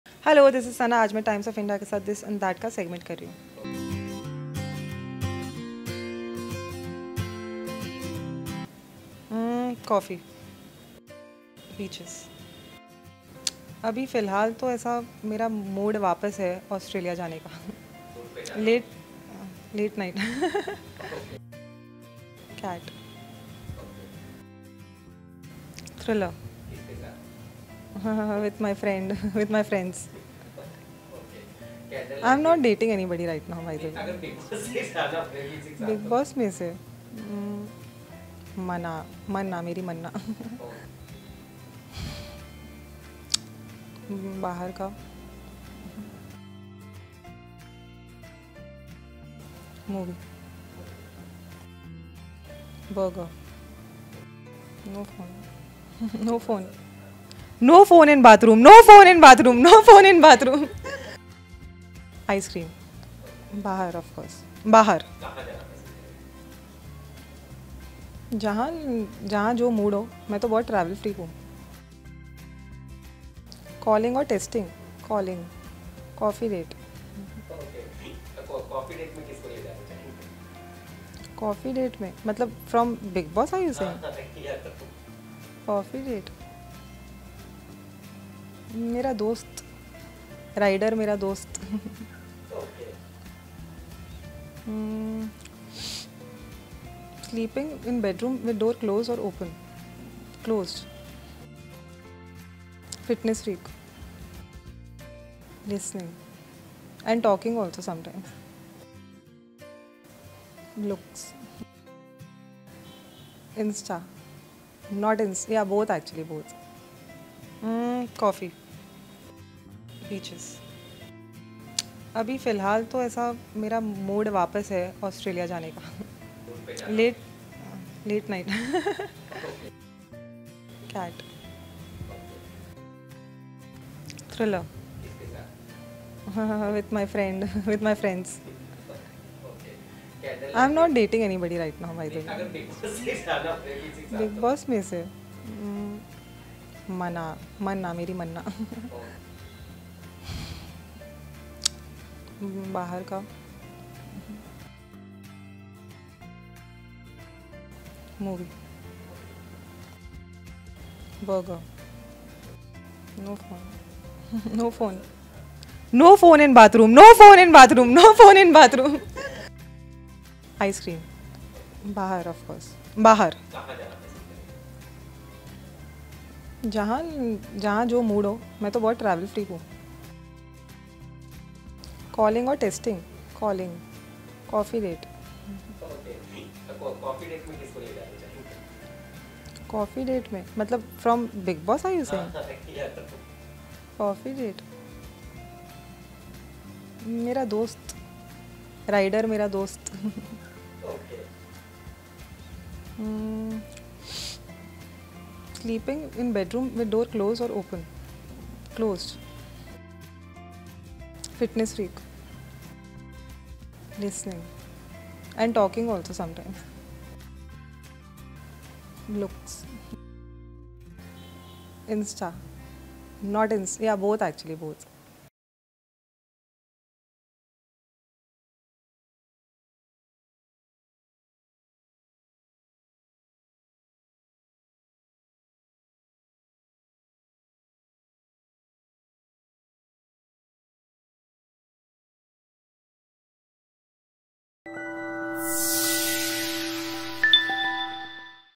This or that ft. Bigg Boss 17's Sana Raees Khan; I am not dating anyone
Sana Raees Khan who was last seen in Bigg Boss 17 played a fun segment This Or That with ETimes TV. From her favourite call time for the shoot to favourite beverage, Sana made these interesting revelations from her personal life. She also opened up about her food choices. Watch the video to know more.